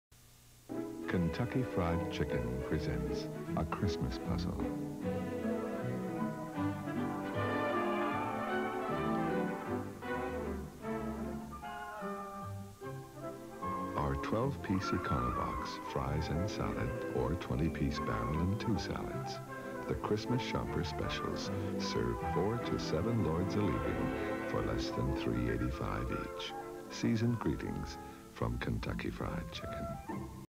KFC Christmas commercial 1989
An old KFC commercial from 1989 Christmas holidays.